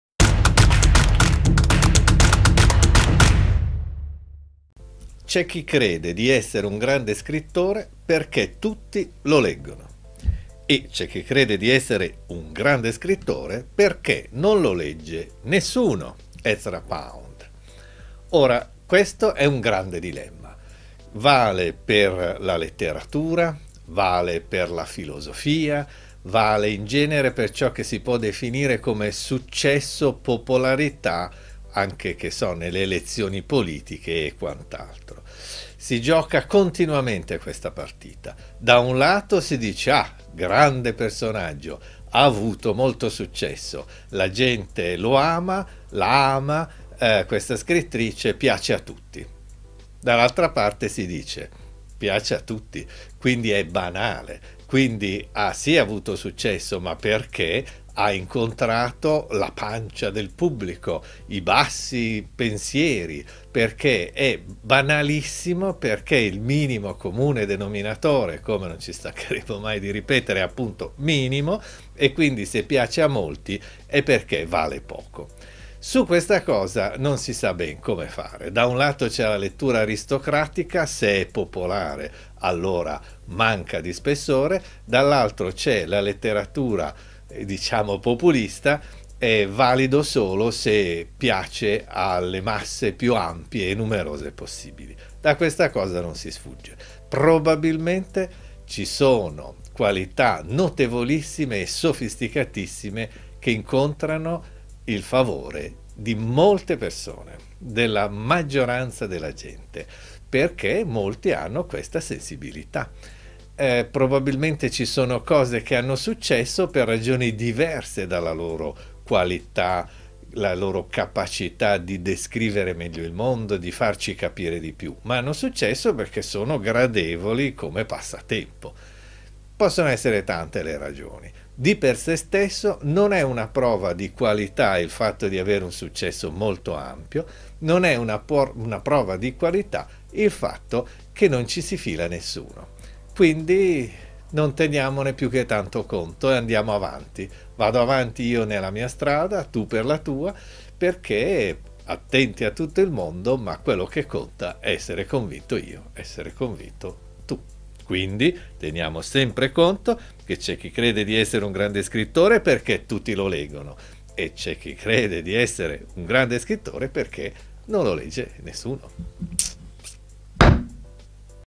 C'è chi crede di essere un ...
C'è chi crede di essere un grande scrittore perché tutti lo leggono e c'è chi crede di essere un grande scrittore perché non lo legge nessuno
@ Ezra Pound

Per attingere a tutte le citazioni che ci ispirano ad ESSERCI sempre al meglio, vai subito alla playlist della Serie: